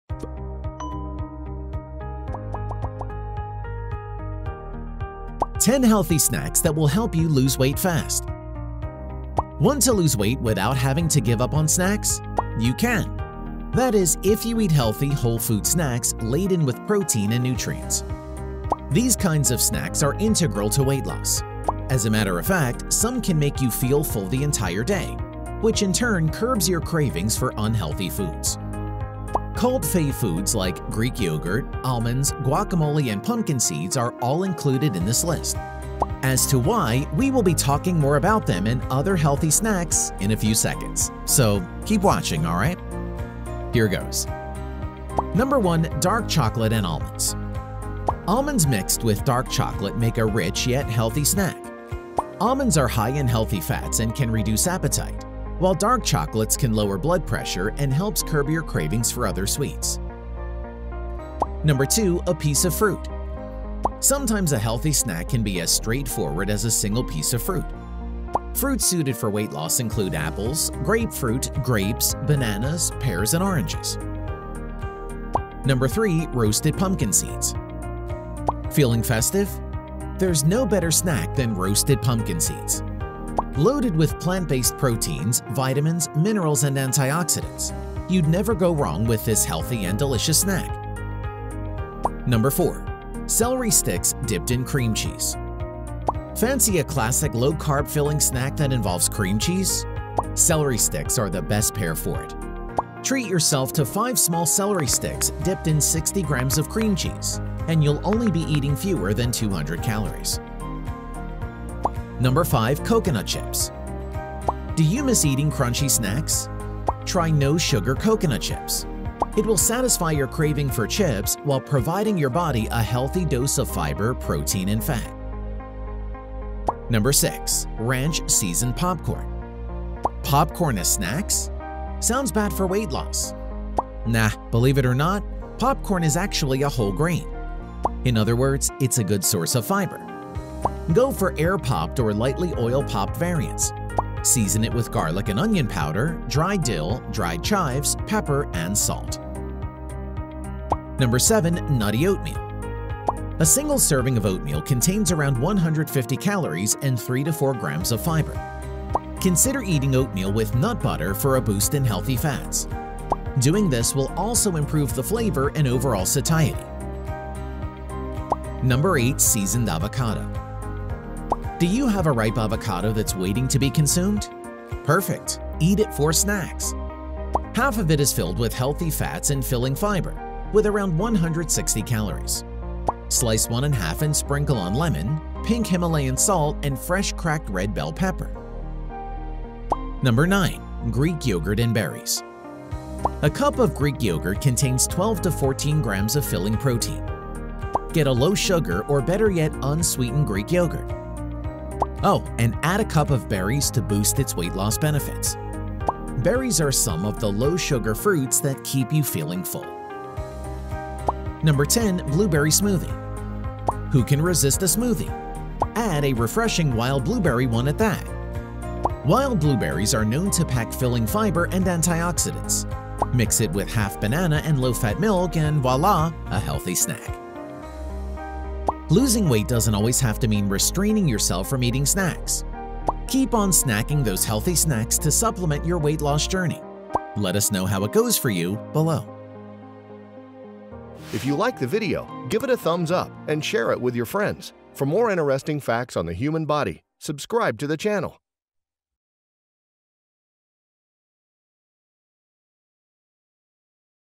10 Healthy Snacks That Will Help You Lose Weight Fast
Want to lose weight without having to give up on snacks?

You can! That is if you eat healthy, whole-food snacks laden with protein and nutrients.

These kinds of snacks are integral to weight loss.

As a matter of fact, some can make you feel full the entire day, which in turn, curbs your cravings for unhealthy foods.

Cult-fave foods like Greek yogurt, almonds, guacamole, and pumpkin seeds are all included in this list.

As to why, we will be talking more about them and other healthy snacks in a few seconds, so keep watching, alright? Here goes.

1. Dark chocolate and almonds
Almonds mixed with dark chocolate make a rich yet healthy snack.

Almonds are high in healthy fats and can reduce appetite while dark chocolates can lower blood pressure and helps curb your cravings for other sweets.

2. A piece of fruit
Sometimes a healthy snack can be as straightforward as a single piece of fruit.

Fruits suited for weight loss include apples, grapefruit, grapes, bananas, pears, and oranges.

3. Roasted pumpkin seeds
Feeling festive? There’s no better snack than roasted pumpkin seeds.

Loaded with plant-based protein, vitamins, minerals, and antioxidants, you’d never go wrong with this healthy and delicious snack.

4. Celery sticks dipped in cream cheese
Fancy a classic low-carb, filling snack that involves cream cheese?

Celery sticks are the best pair for it.

Treat yourself to five small celery sticks dipped in 60 grams of cream cheese, and you’ll only be eating fewer than 200 calories.

5. Coconut chips
Do you miss eating crunchy snacks?

Try no-sugar coconut chips.

It will satisfy your craving for chips while providing your body a healthy dose of fiber, protein, and fat.

6. Ranch seasoned popcorn
Popcorns as snacks? Sounds bad for weight loss?

Nah, believe it or not, popcorn is actually a whole grain.

In other words, it’s a good source of fiber.

Go for air-popped or lightly oil-popped variants.

Season it with garlic and onion powder, dried dill, dried chives, pepper, and salt.

7. Nutty oatmeal
A single serving of oatmeal contains around 150 calories and 3-4 grams of fiber.

Consider eating oatmeal with nut butter for a boost in healthy fats.

Doing this will also improve the flavor and overall satiety.

8. Seasoned avocado
Do you have a ripe avocado that’s waiting to be consumed? Perfect! Eat it for snacks.

Half of it is filled with healthy fats and filling fiber with around 160 calories.

Slice one in half and sprinkle on lemon, pink Himalayan salt, and fresh cracked red bell pepper

9.Greek yogurt and berries
A cup of Greek yogurt contains 12-14 grams of filling protein.

Get a low-sugar, or better yet, unsweetened Greek yogurt.

Oh, and add a cup of berries to boost its weight loss benefits.

Berries are some of the low-sugar fruits that keep you feeling full.

10. Blueberry smoothie
Who can resist a smoothie?

And a refreshing wild blueberry one at that!

Wild blueberries are known to pack filling fiber and antioxidants.

Mix it with half banana, and low-fat milk, and voila--a healthy snack.

Losing weight doesn’t always have to mean restraining yourself from eating snacks.

Keep on snacking those healthy snacks to supplement your weight loss journey.

Let us know how it goes for you below. -